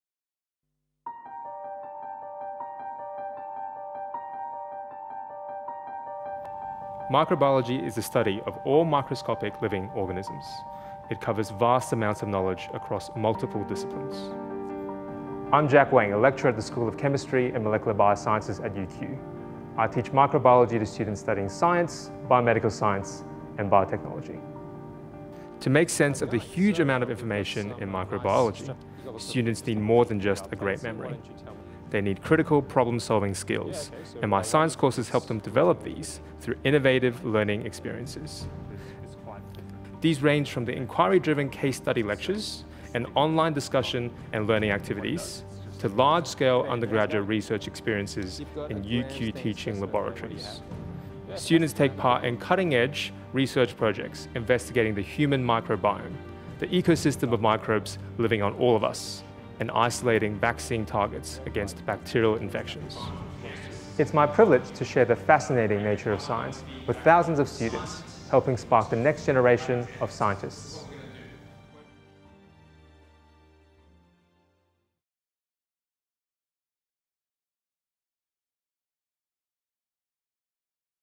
Jack Wang
Dr Jack Wang is an early career lecturer in microbiology at UQ. Since 2010, he has coordinated courses in medical and molecular microbiology and serves as the microbiology major convenor. His work focuses on integrating the scientific method into all aspects of science education to drive the development of critical thinking skills in large student cohorts. In collaboration with leaders in microbiology and education, Dr Wang has integrated research-led learning experiences to a framework of undergraduate courses across the UQ microbiology major. These learning activities have immersed thousands of students into the inquiry-driven nature of science, while providing opportunities to utilise cutting-edge technology in microbiology research. His students have demonstrated learning gains in transferrable scientific skills as well as high levels of engagement. This work has attracted competitive grant funding, been disseminated in peer-reviewed journals, international conferences, and Office for Learning and Teaching (OLT) good-practice guides, culminating in a 2013 UQ Faculty of Science Teaching Excellence Award. Dr Wang is an emerging early-career academic who has demonstrated leadership in teaching and learning, and is dedicated to advancing science education.